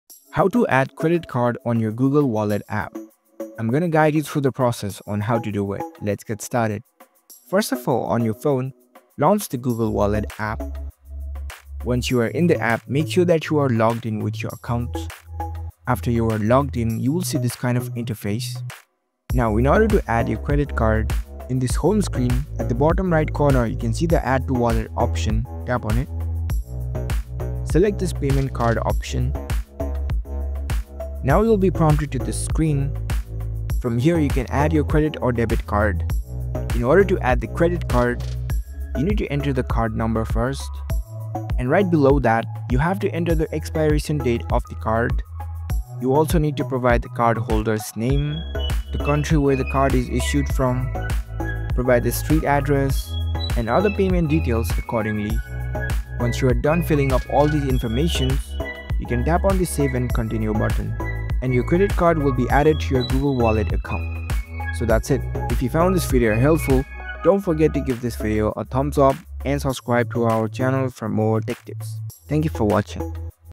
How to Add Credit Card in Google Wallet 2025?
Want to add a credit card to Google Wallet for fast and secure payments? Here’s how! Linking your credit card to Google Wallet allows you to make contactless payments, shop online, and store your card details securely. In this guide, we’ll walk you through adding your credit card via the Google Wallet app, verifying your card for payments, and troubleshooting issues if the card isn’t adding properly. Set up your card in minutes and enjoy seamless transactions!